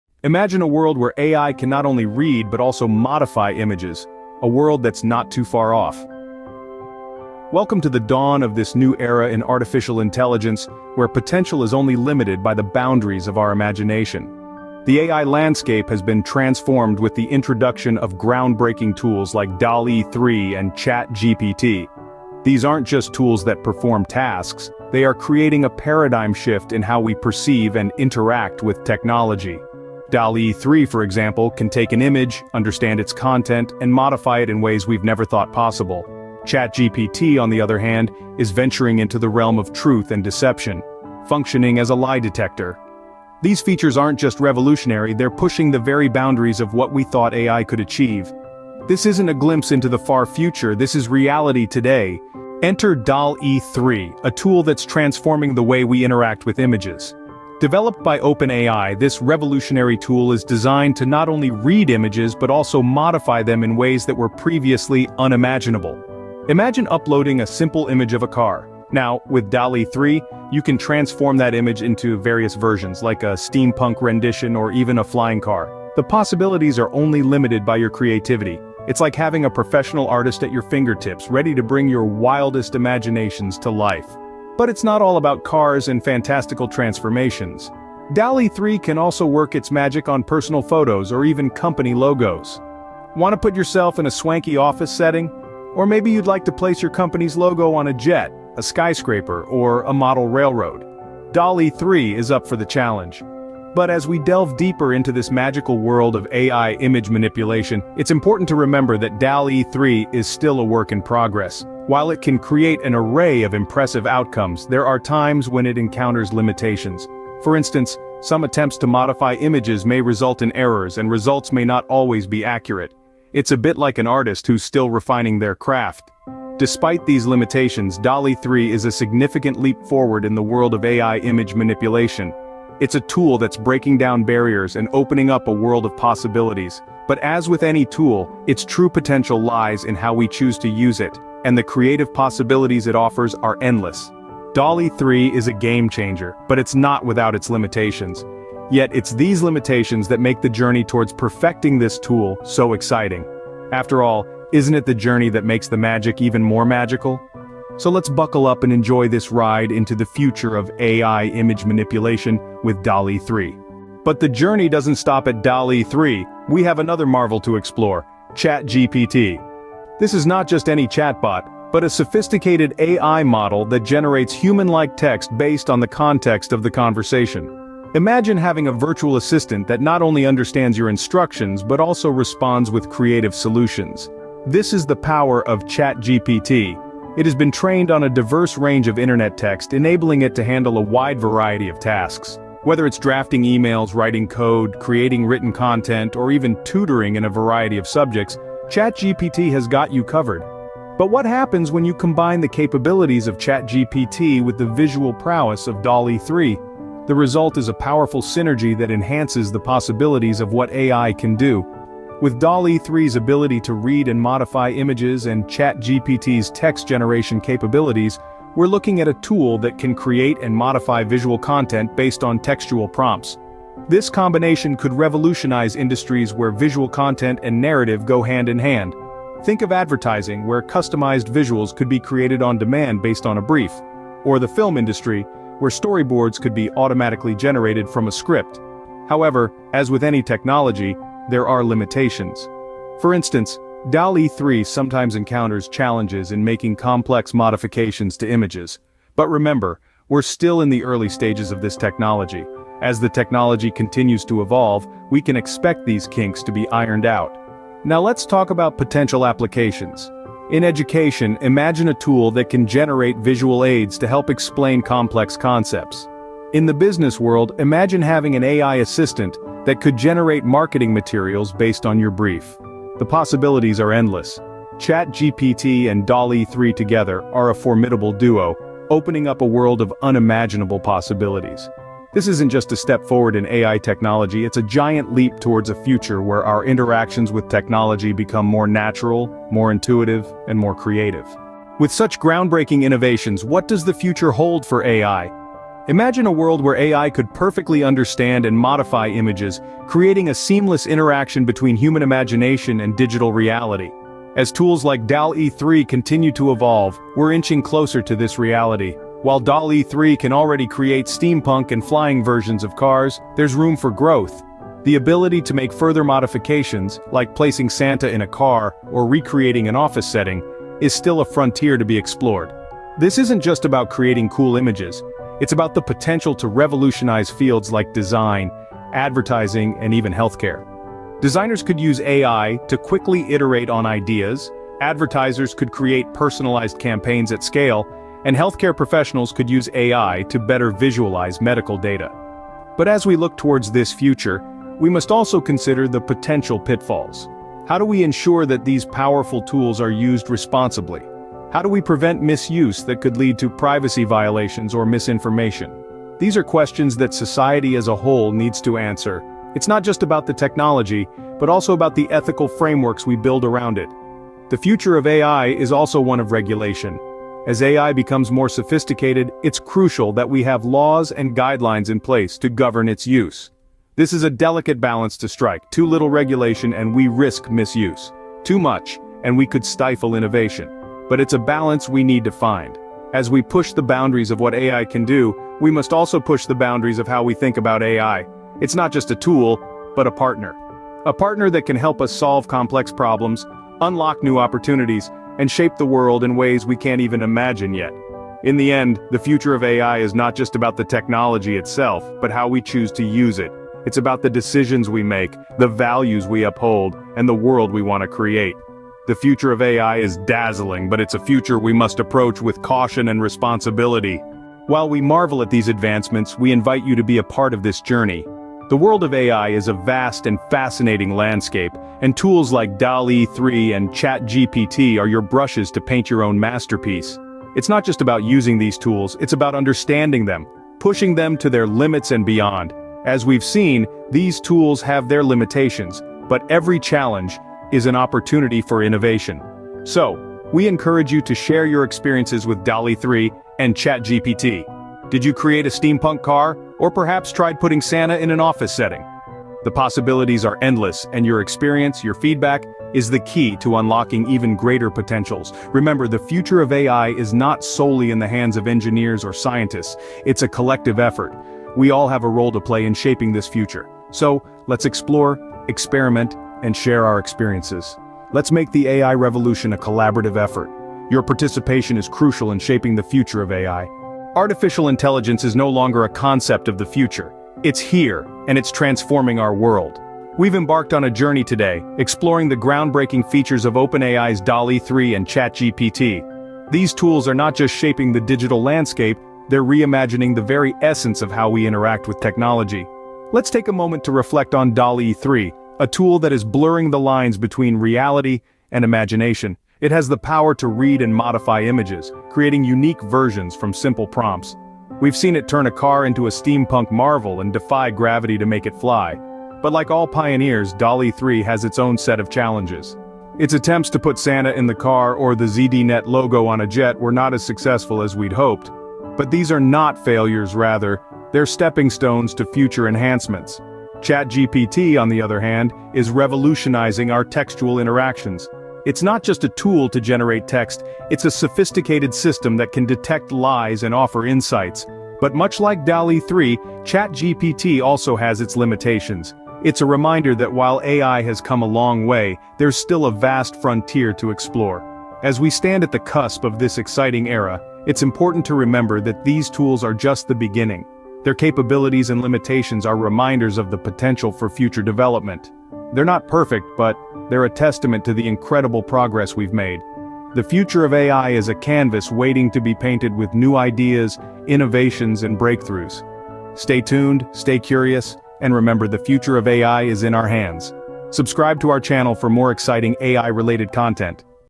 The Future Unleashed: OpenAI's Vision and Game-Changing Breakthroughs
In this video, we're unveiling OpenAI's brand new DALL-E 3 machine learning algorithm and chatgpt advanced data analysis tool!

DALL-E 3 is a powerful machine learning algorithm that can detect deception in a conversation. With this new tool, you can explore how chatgpt users communicate and interact with each other. So be sure to check out this video to learn more about DALL-E 3 and chatgpt!